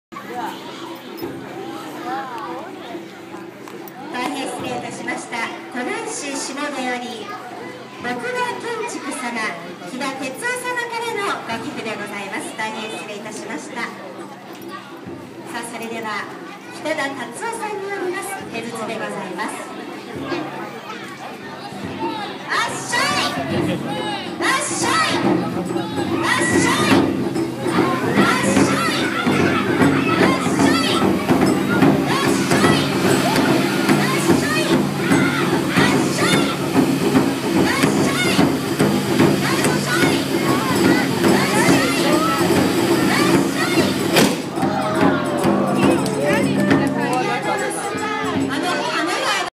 2013 矢川神社「七夕まつり」 甲賀手筒花火その１
2013年７月７日に滋賀県甲賀市にある矢川神社で行われた「七夕まつり」での甲賀手筒花火の模様です。

珍しく自分でiphone手持ち撮影しています（笑）